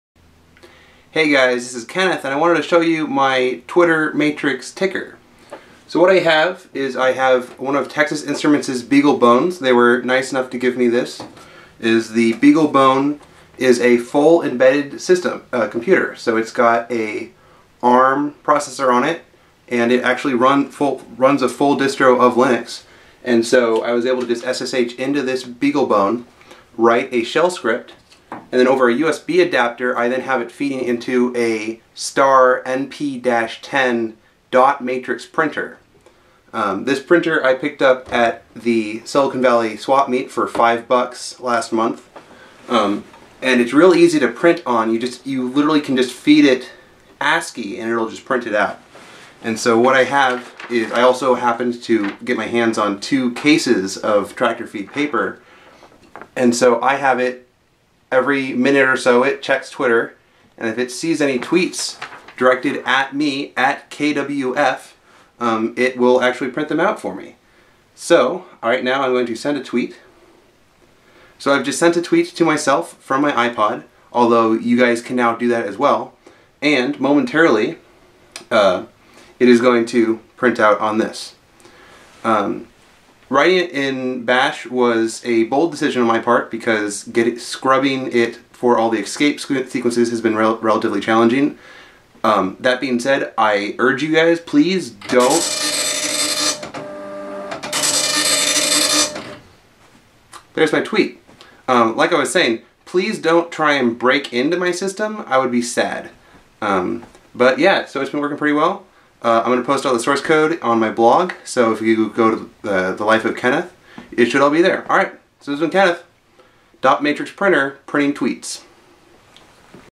TwitterMatrixTicker Demo
Using a BeagleBone and a dot matrix printer, I can now be just as hip as everyone else with their Twitter to paper and ink devices.